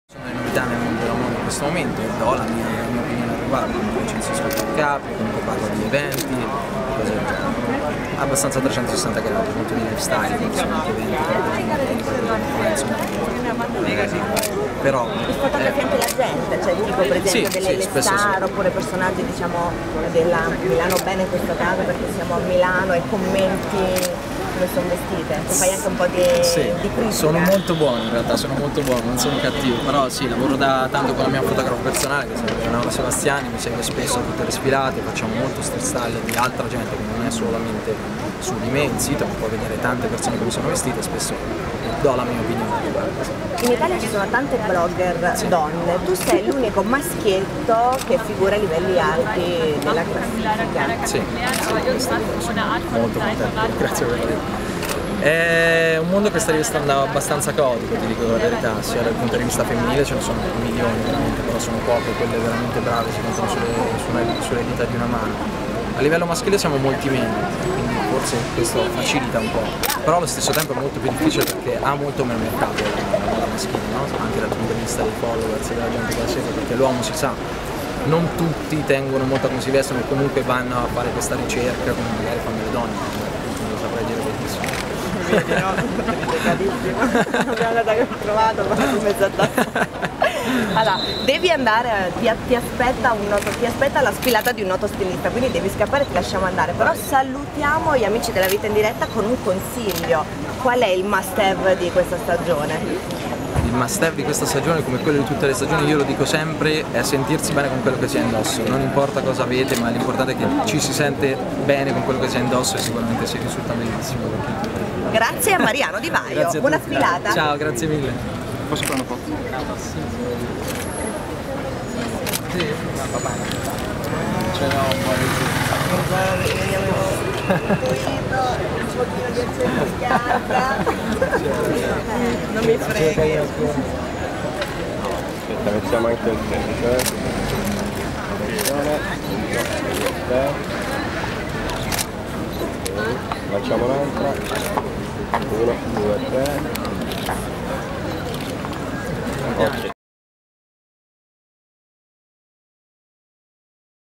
MARIANO DI VAIO @ Milano Fashion Week
Mariano Di Vaio interviene nel corso della settimana della moda di Milano.